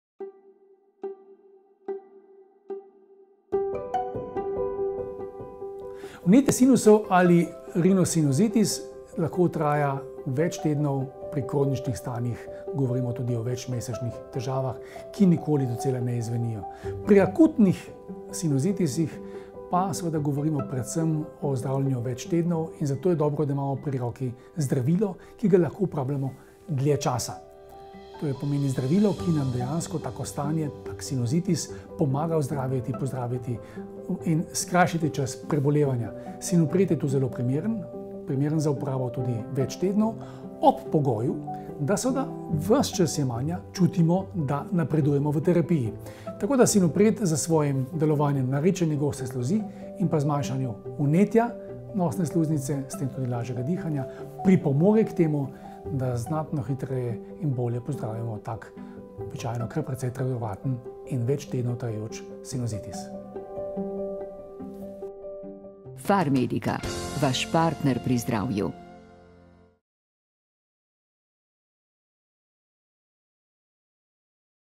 Dolgoročno jemanje Sinupreta
Vnetje sinusov lahko traja ... tedne, mesece. Terapija s Sinupretom vam pomaga na dolgi rok. Prisluhnite prispevku.